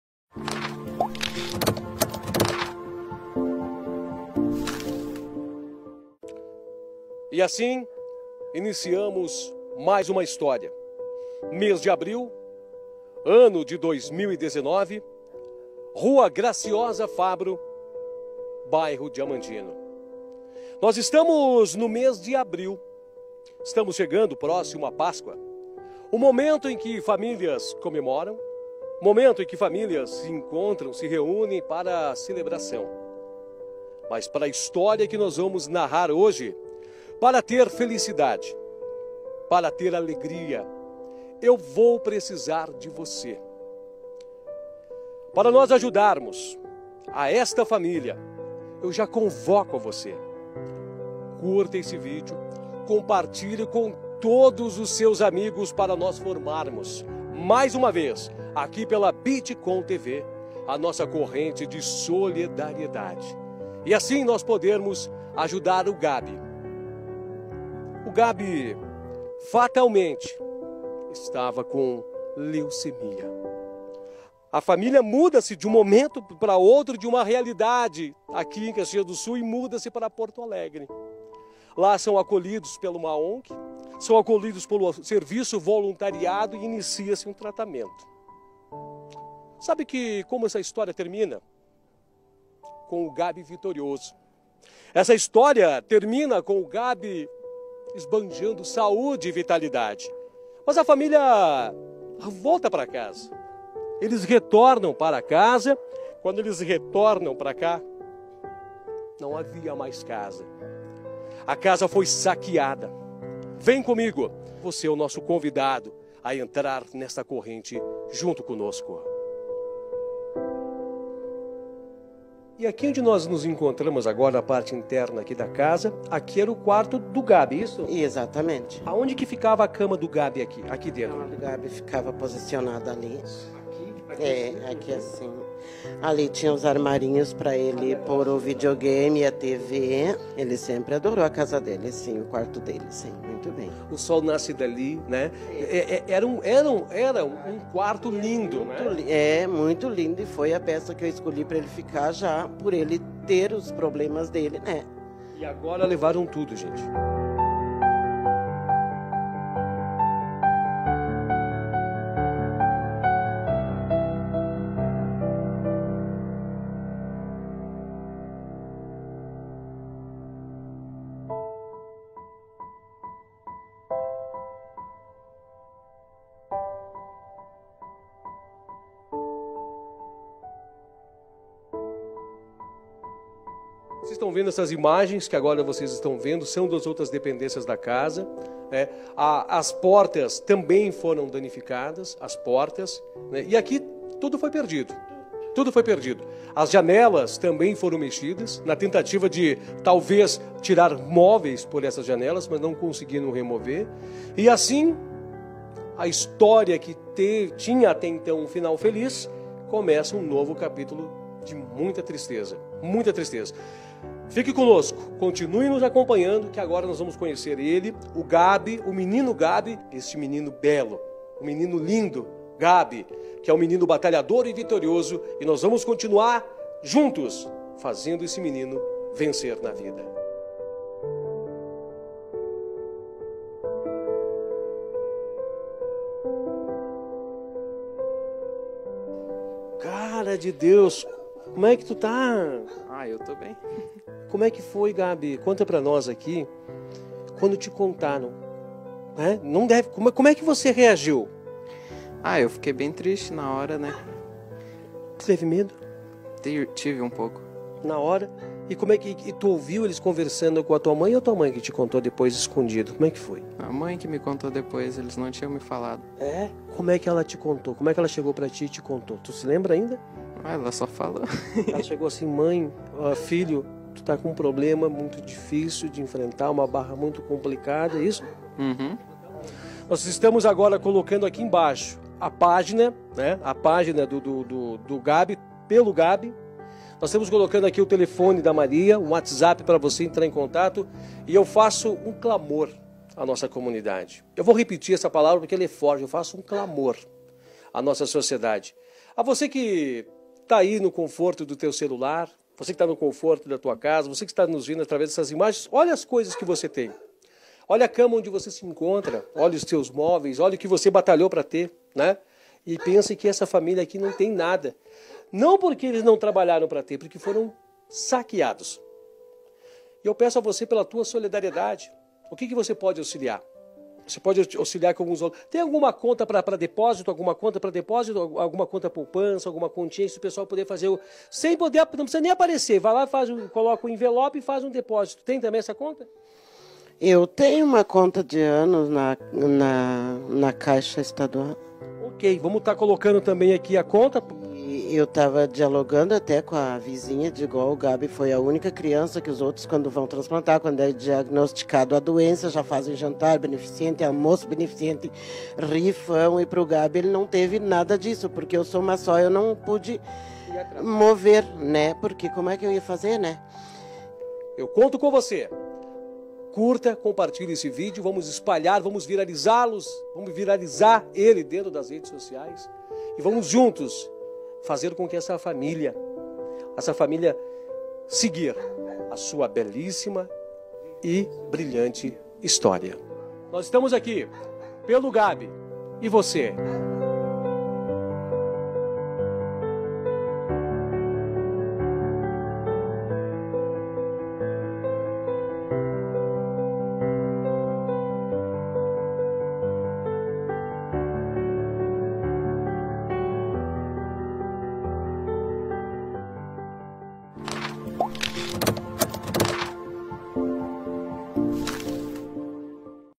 Pelos bairros com Edinho Soares
Conheça a história do Gabi que mora no bairro Diamantino e precisa da sua ajuda.